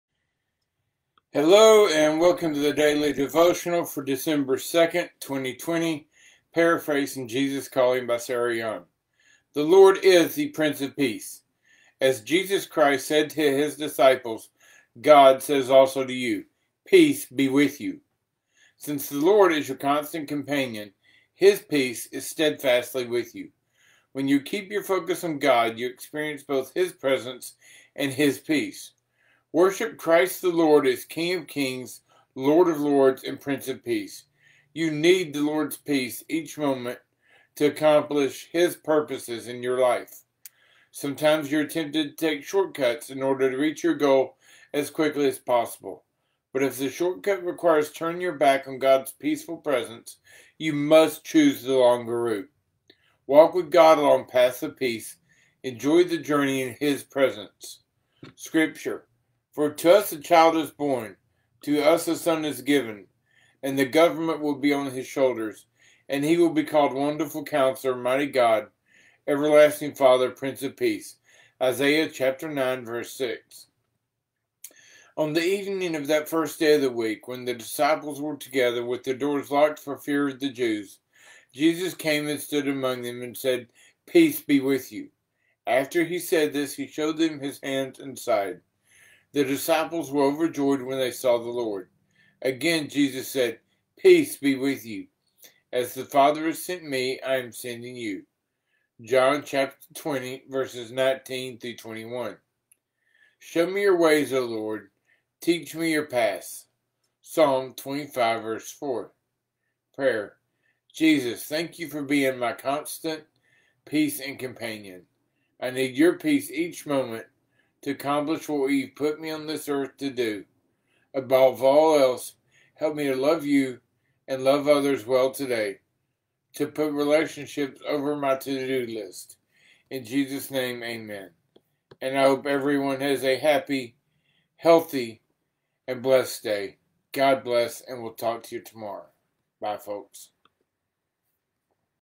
THE LORD IS THE PRINCE OF PEACE.
"THE LORD IS THE PRINCE OF PEACE. As Jesus Christ said to HIS disciples, GOD says also to you: Peace be with you. Since The LORD is your constant Companion, HIS Peace is steadfastly with you. When you keep your focus on GOD, you experience both HIS Presence and HIS Peace. Worship Christ The LORD as King of kings, Lord of lords, and Prince of Peace. You need The LORD'S Peace each moment to accomplish HIS purposes in your life. Sometimes you are tempted to take shortcuts, in order to reach your goal as quickly as possible. But if the shortcut requires turning your back on GOD'S peaceful Presence, you must choose the longer route. Walk with GOD along paths of Peace; enjoy the journey in HIS Presence."  para-phrased -  "Jesus Calling" by Sarah Young

For to us a child is born, to us a son is given, and the government will be on his shoulders. And he will be called Wonderful Counselor, Mighty God, Everlasting Father, Prince of Peace. ISAIAH 9 : 6

On the evening of that first day of the week, when the disciples were together, with the doors locked for fear of the Jews, Jesus came and stood among them and said, “Peace be with you!” After he said this, he showed them his hands and side. The disciples were overjoyed when they saw the Lord. Again Jesus said, “Peace be with you! As the Father has sent me, I am sending you.” JOHN 20 : 19 – 21

Show me Your ways, O LORD; Teach me Your paths. PSALM 25 : 4 (NKJV)

Prayer: Jesus thank you for being my constant peace and companion. I need your peace each moment to accomplish what you've put me on this earth to do. Above all else, help me to love you and love others well today. To put relationships over my to do list. In Jesus' name. Amen.